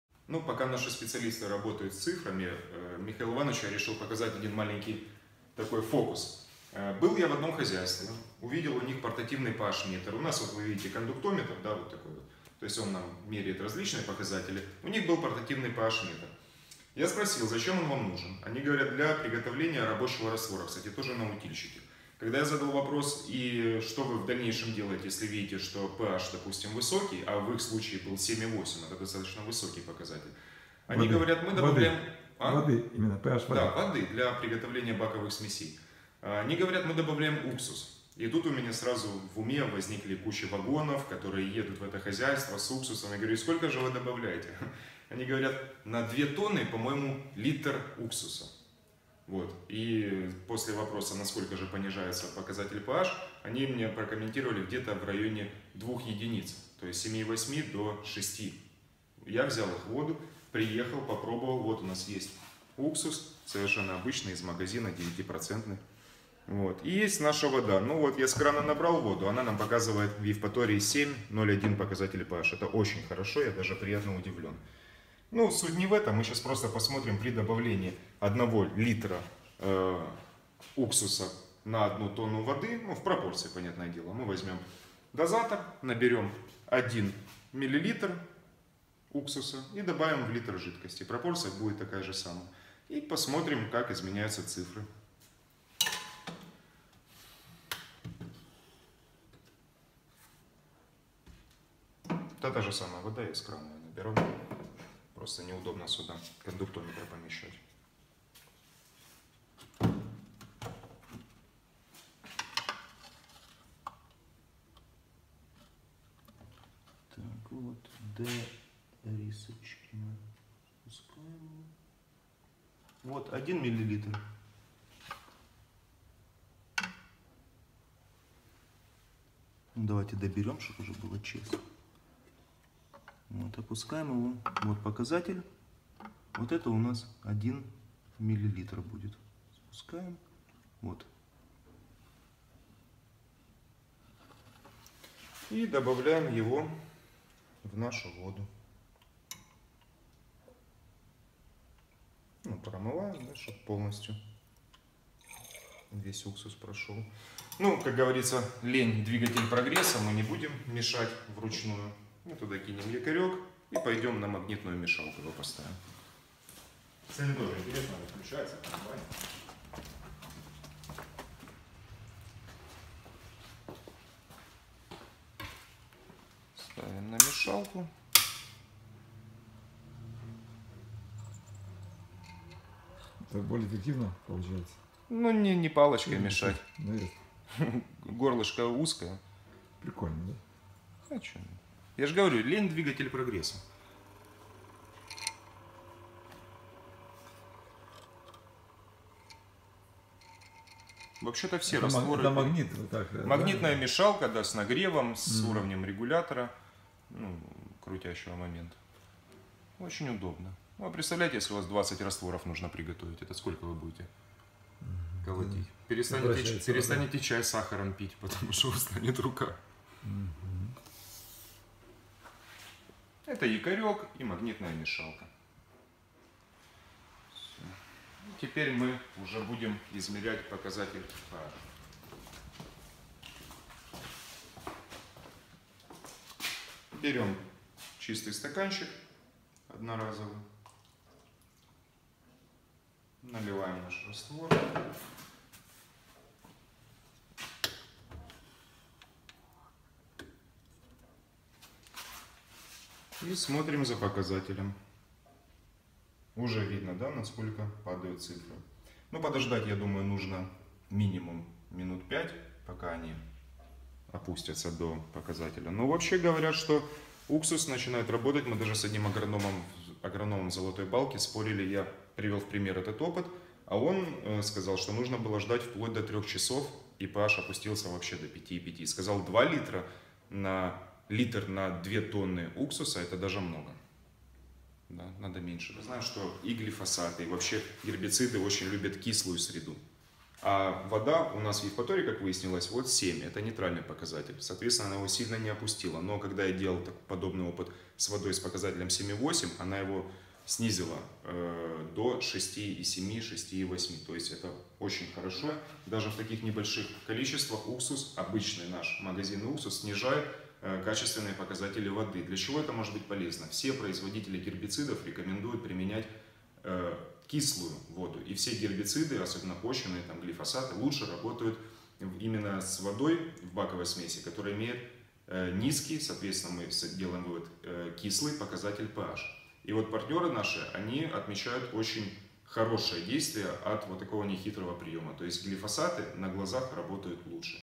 Простой способ улучшить воду для опрыскивания
Арсений Бордзидский (компания Сана Микс, Евпатория) наглядно продемонстрировал, как можно улучшить воду для опрыскивания.